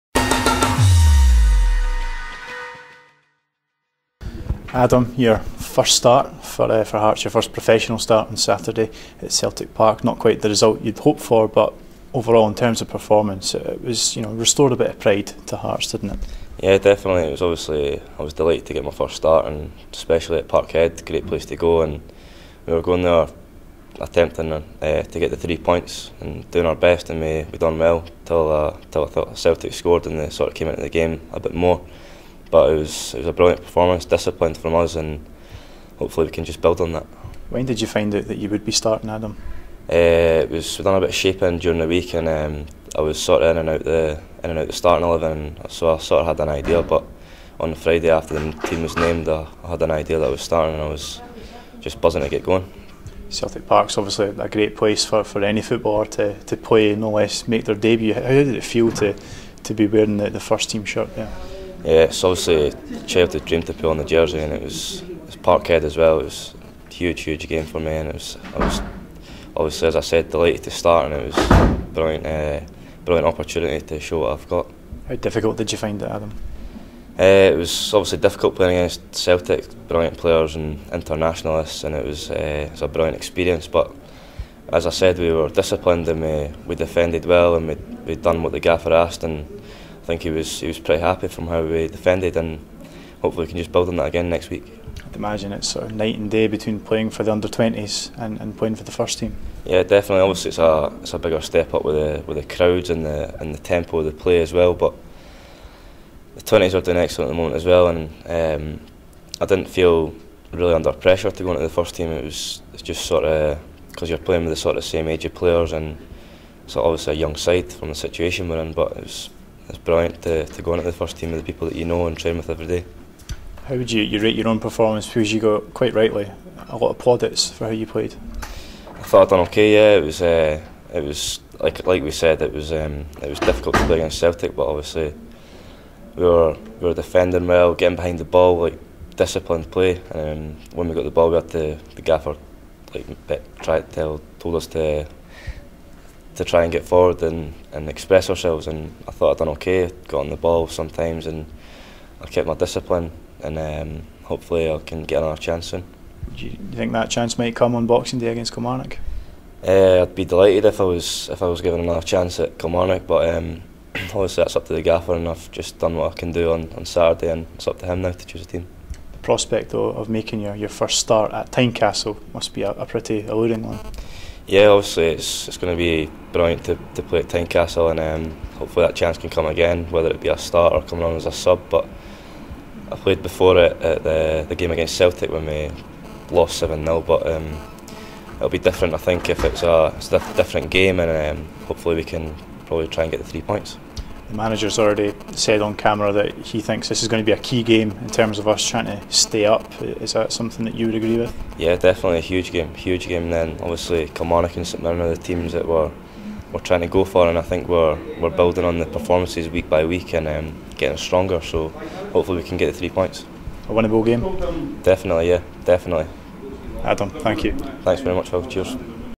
HeartsTV: Adam speaks
Adam King gives his first HeartsTV interview in this free-to-view video. Merry Christmas!